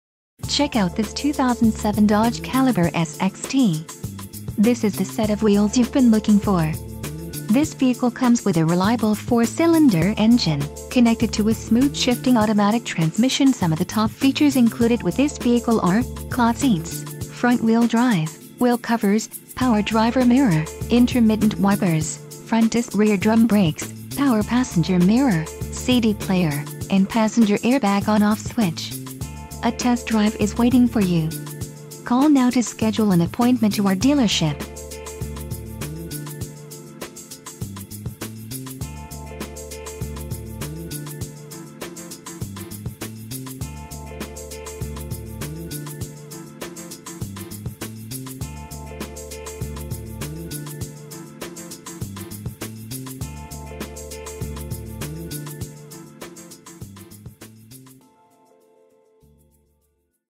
2007 Dodge Caliber - To Schedule a Test Drive - Phoenix, AZ 85019
Check out this 2007 Dodge Caliber Sxt. This is the set of wheels you've been looking for. This vehicle comes with a reliable 4 cylinder engine, connected to a smooth shifting automatic transmission.. Some of the top features included with this vehicle are: Cloth Seats, Front Wheel Drive, Wheel Covers, Power Driver Mirror, Intermittent Wipers, Front Disc/Rear Drum Brakes, Power Passenger Mirror, CD Player, and Passenger Air Bag On/Off Switch. A test drive is waiting for you.